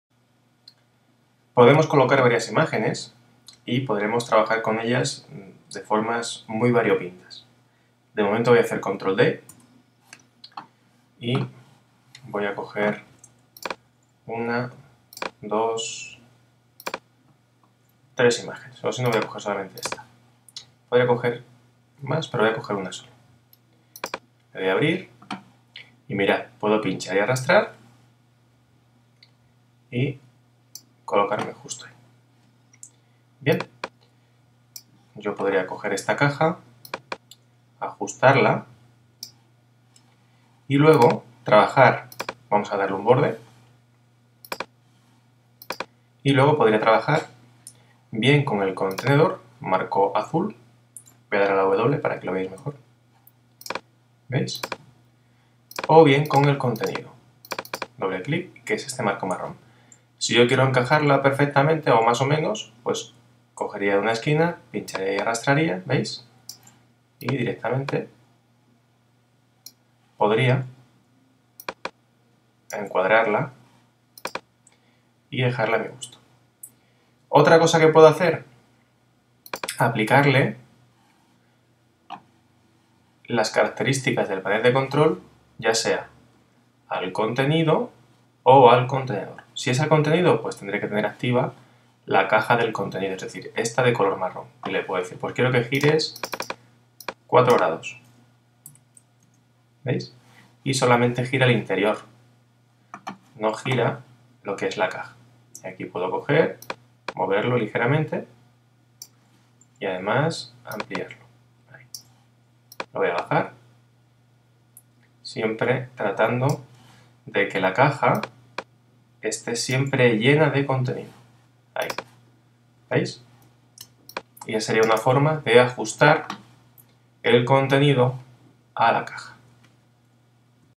37 Ajustar contenido a caja
Debemos ser muy ordenados y asignar a cada caja un espacio que debe coincidir con el contenido que albergan. Si no quieres hacer el proceso de forma manual, sólo tienes que ver es vídeo.